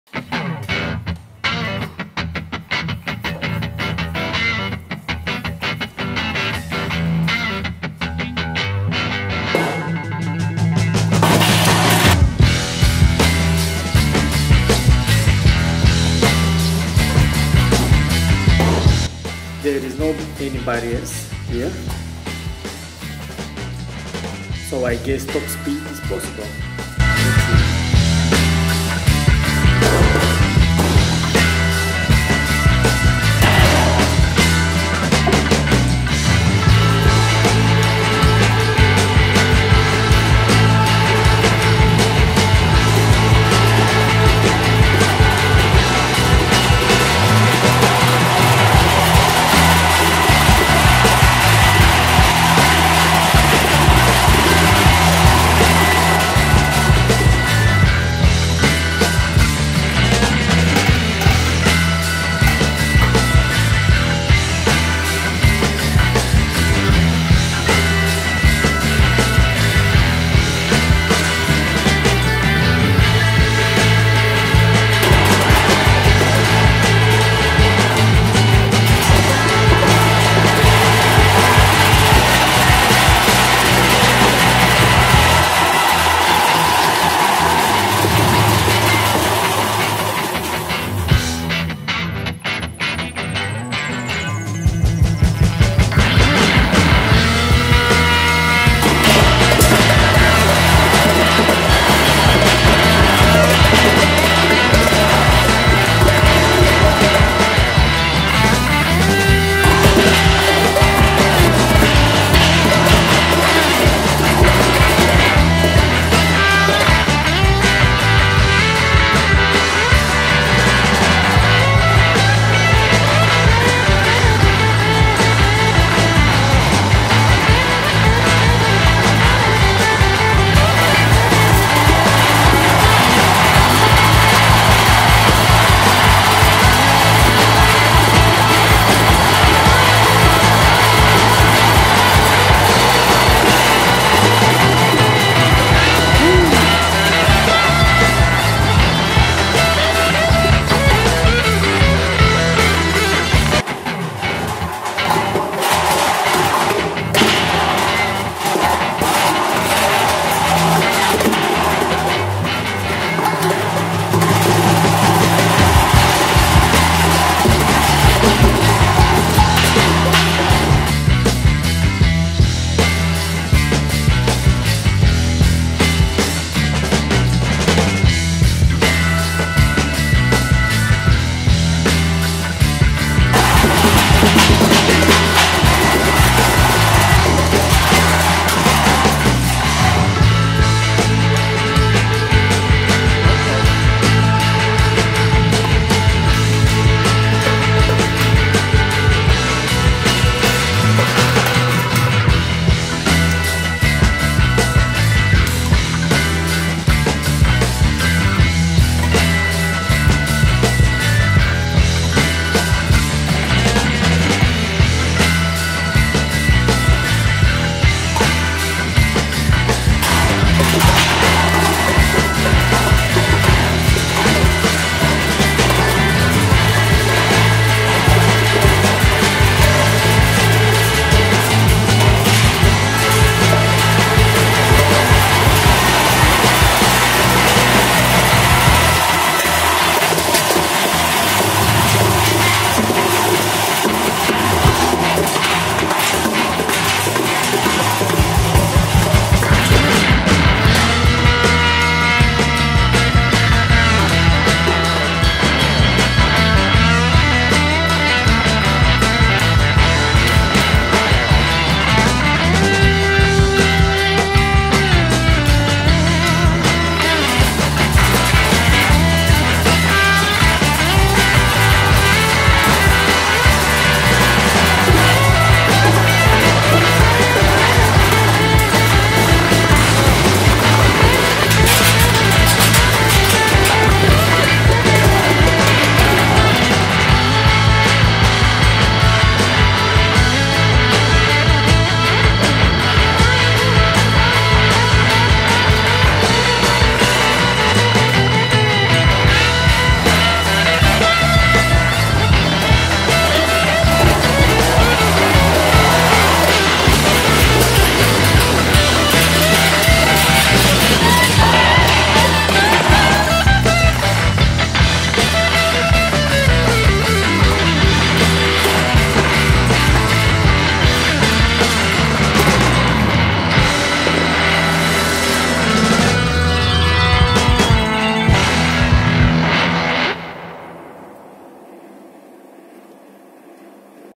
Experiment - Bowling Ball on Top Speed - in a Washing Machines
great speeds, damage and wobble